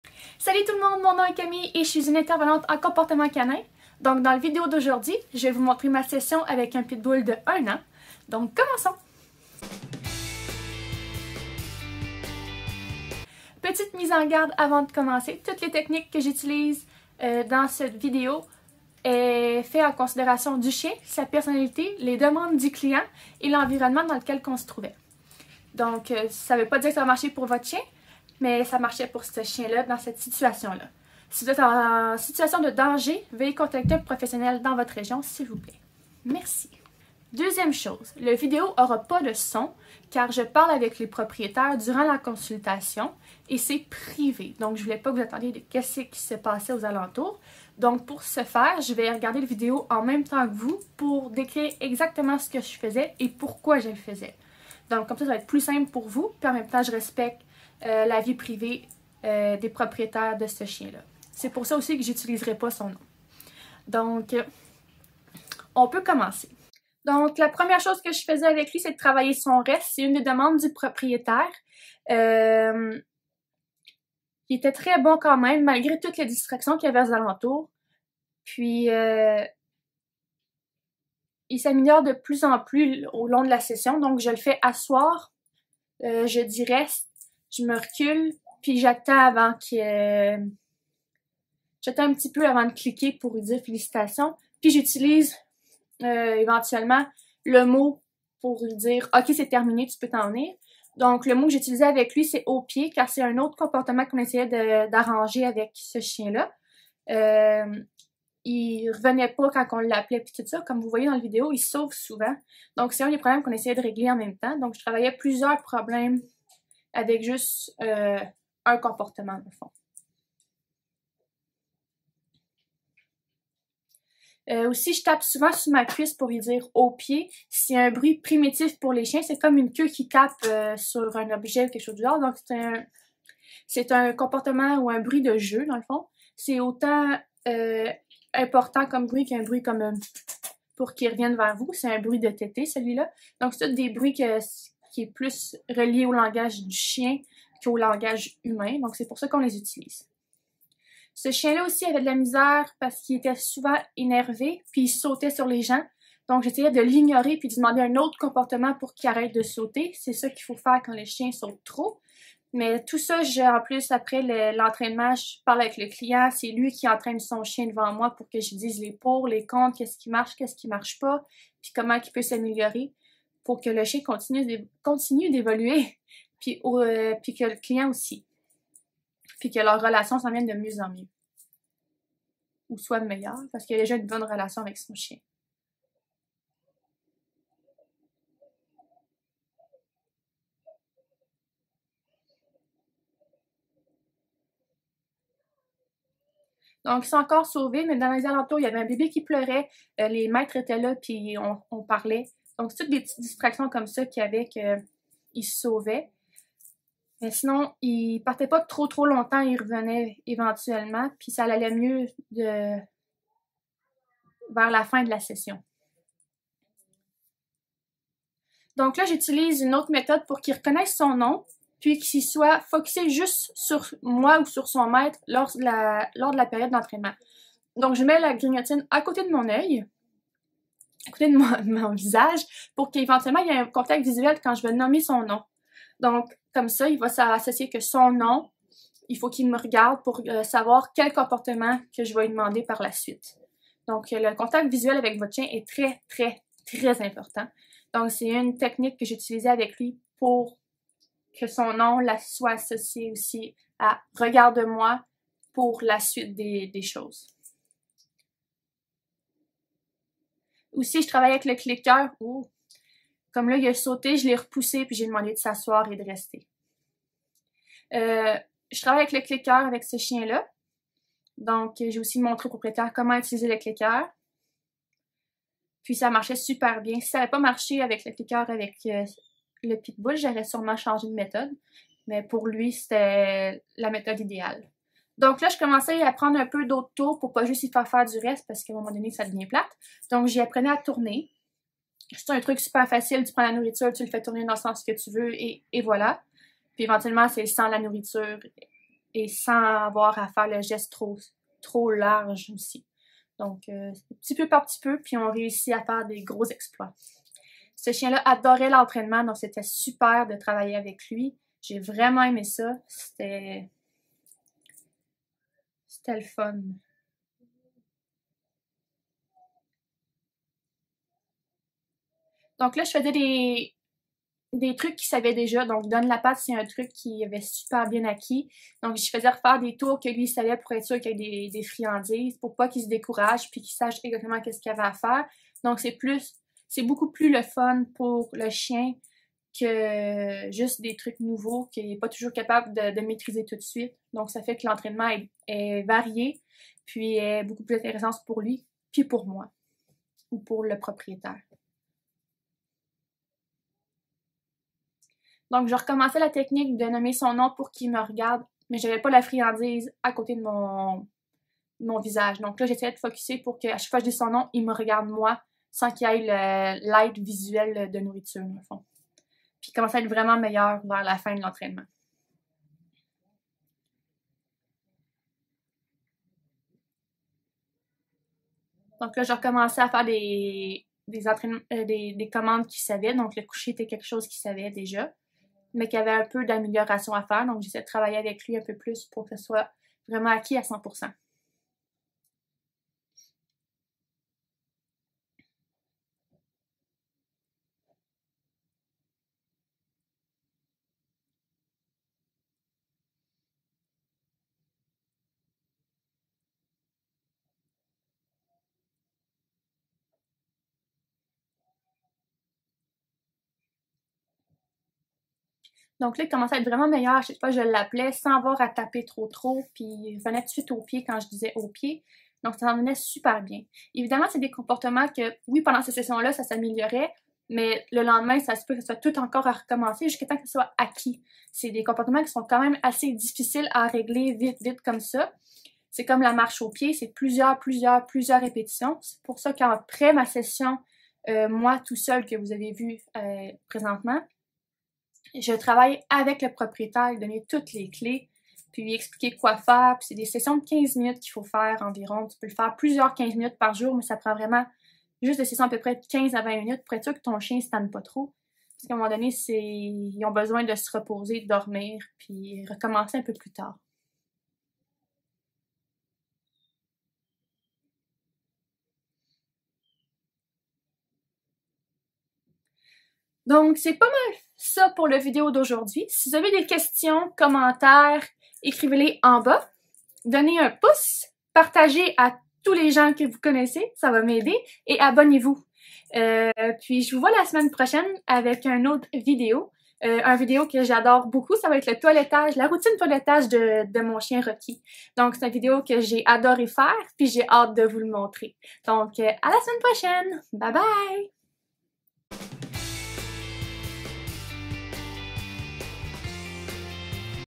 Cours de dressage avec un Pitbull --- Camille L'Étoile ---
Bonjour

Je suis intervenante en comportement et dans le video d'aujourd'hui je vous montre une partie d'une consultation de dressage avec un beau Pitbull de 1 an.

Si vous avez question commentaire vous pouvez me joindre dans les commentaires.

Merci et a la semaine prochaine.

Camille